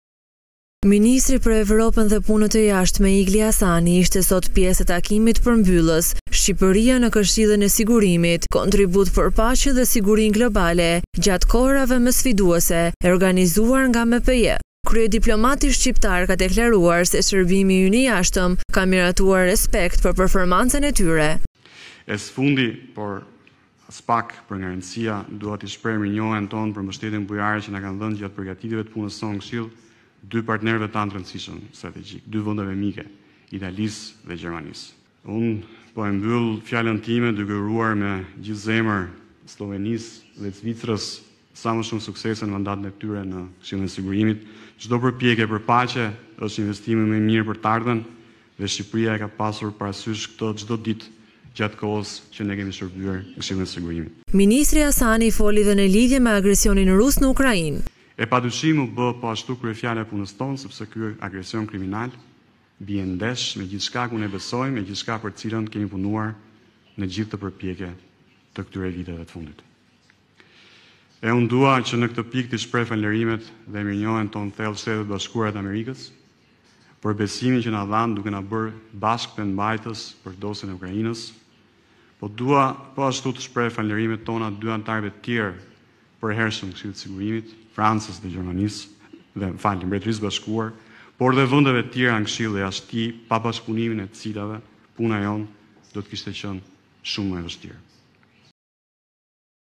Kohë e vështirë për Botën/ Ministri Hasani për agresionin në Ukrainë
Ministri për Evropën dhe Punët e Jashtme, Igli Hasani, ishte sot pjesë e takimit përmbyllës “Shqipëria në Këshillin e Sigurimit, kontribut për paqen dhe sigurinë globale gjatë kohërave më sfiduese” e organizuar nga MEPJ.
Kryediplomati shqiptar ka deklaruar se shërbimi ynë i Jashtëm ka merituar respekt për performancën e tyre.
Ministri Hasani foli edhe në lidhje me agresionin rus në Ukrainë.

𝐀𝐮𝐭𝐨𝐫 : NTV